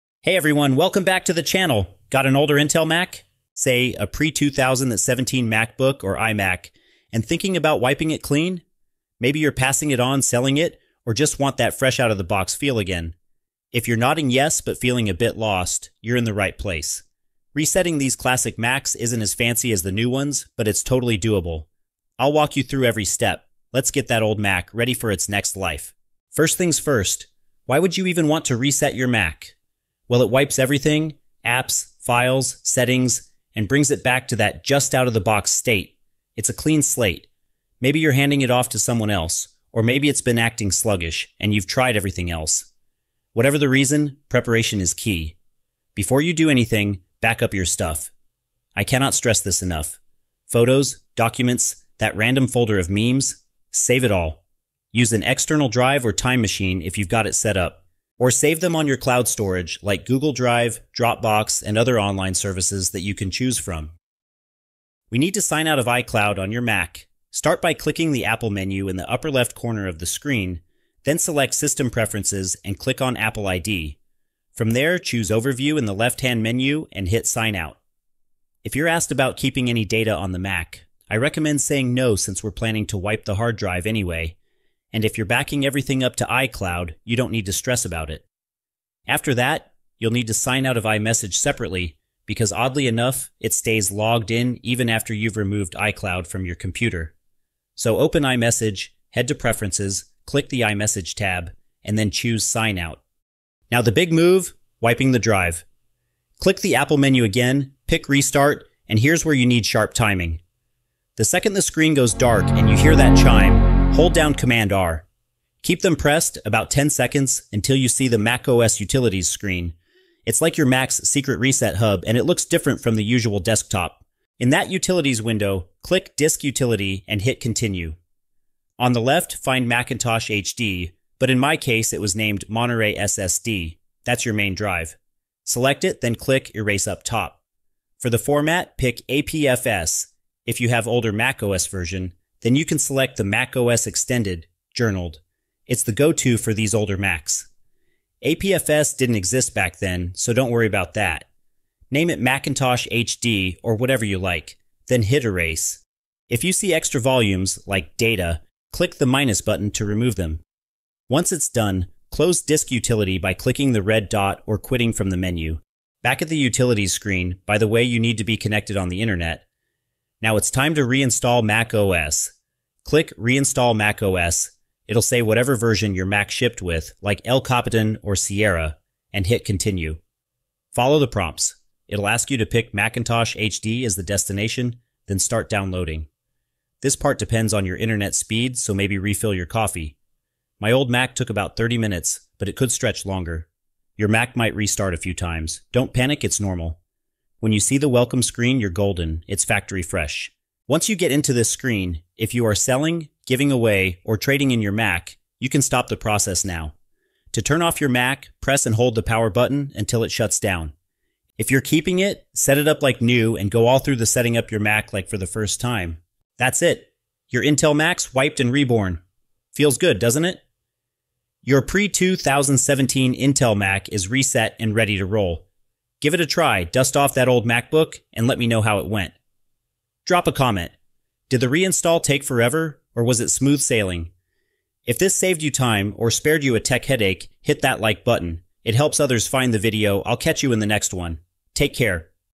How to Factory Reset Your Intel Mac (Pre 2017 Models)
Ready to factory reset your Intel Mac from before 2017? This guide walks you through wiping your old MacBook or iMac—perfect for selling, gifting, or fixing glitches. We cover signing out, erasing the drive, and reinstalling macOS the classic way, all with clear steps for pre-2017 models. It’s your go-to Intel Mac reset tutorial!

Like if this helped, and tell us your Mac story below!